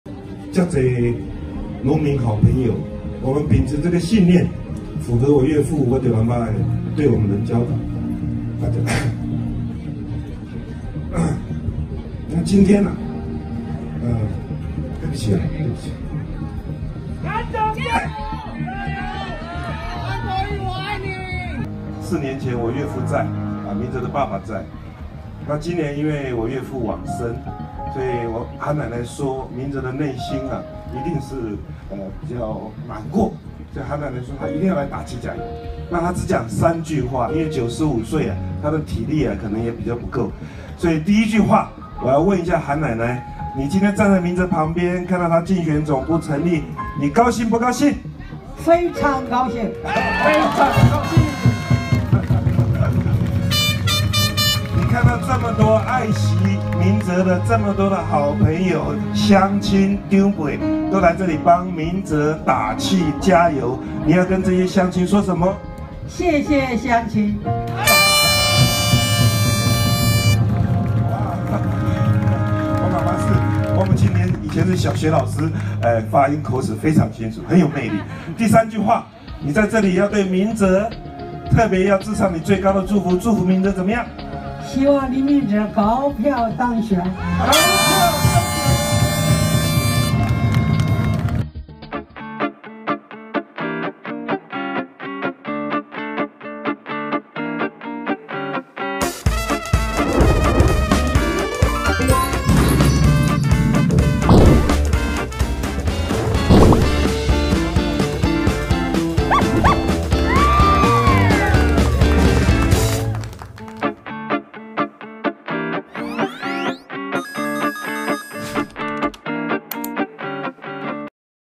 李明哲競總成立 韓國瑜夫妻與韓奶奶相挺！近3千人擠滿會場
記者 蔡佩旻 拍攝